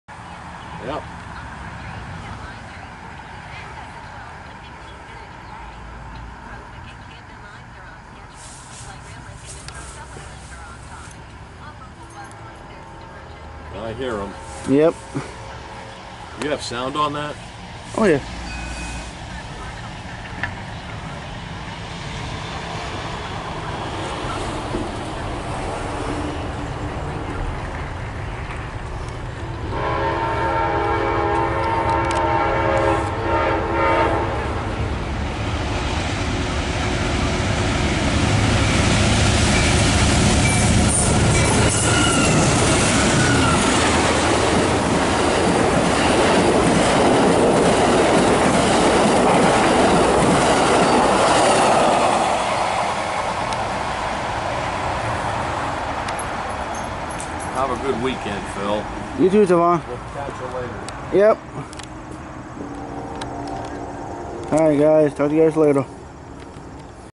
#1632 Amtrak PO29
Amtrak PO29 is heading west on track 1. PO29 came from Washington Union station [DC] to Chicago, IL.